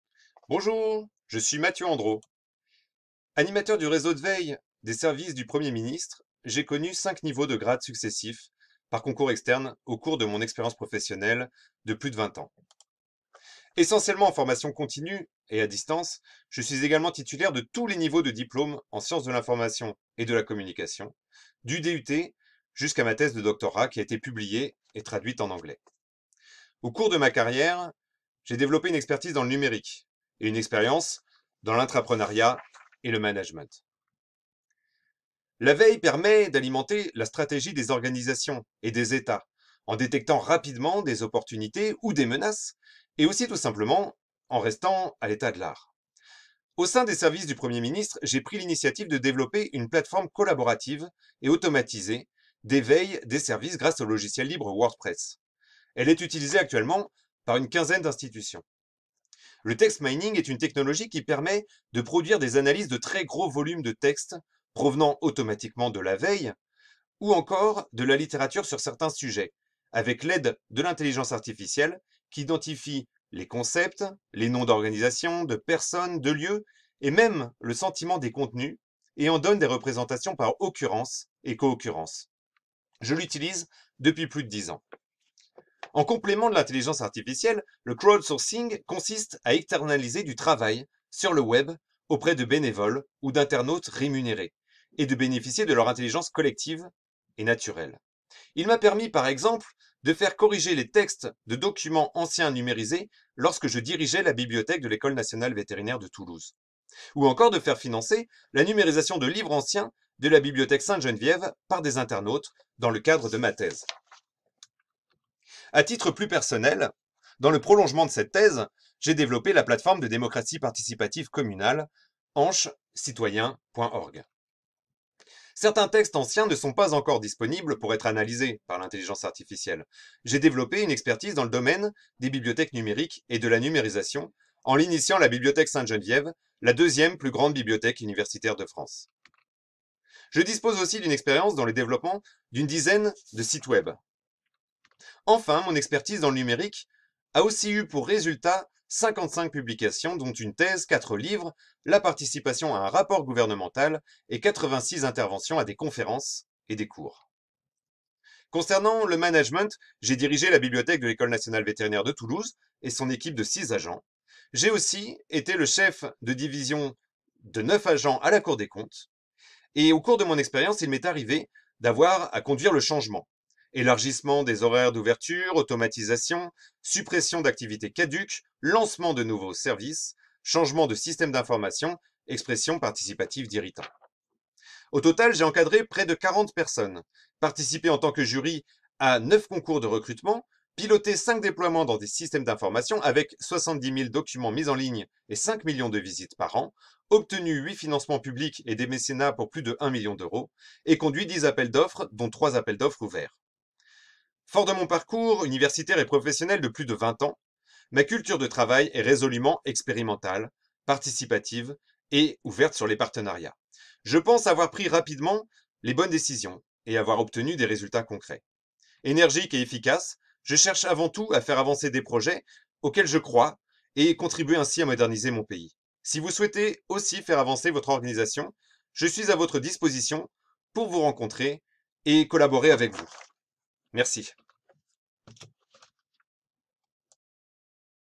CV vidéo de Mathieu Andro, intrapreneur dans le numérique
Docteur en sciences de l’information et de la communication (Paris 8, 2016), Mathieu Andro est actuellement animateur du réseau de veilleurs des Services du Premier ministre. Auparavant, il a travaillé pour les bibliothèques du Muséum national d'Histoire naturelle, dirigé celle de l’École Nationale Vétérinaire de Toulouse, conduit les projets de numérisation de la Bibliothèque Sainte-Geneviève, puis développé des services de text mining à l'Institut national de la recherche agronomique avant de devenir chef d’une division spécialisée dans la veille à la Cour des comptes.

Il est l’auteur de plus de 50 publications sur les bibliothèques numériques, le crowdsourcing, le text mining, la veille et l’open access.

Son CV détaillé est disponible sur bibliotheque-numerique.fr